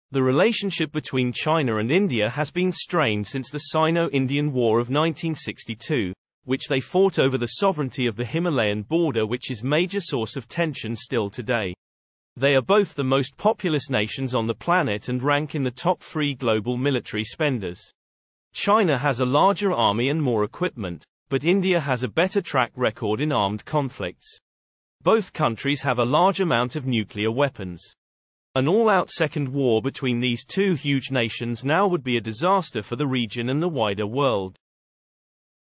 China v India War - Who Wins?
The relationship between China and India has been strained since the Sino-Indian War of 1962, which they fought over the sovereignty of the Himalayan border which is major source of tension still today.
They are both the most populous nations on the planet and rank in the top 3 global military spenders.
China has a larger army and more equipment, but India has a better track record in armed conflicts.
Both countries have a large amount of nuclear weapons.
An all-out between these two nations now would be a disaster for the world.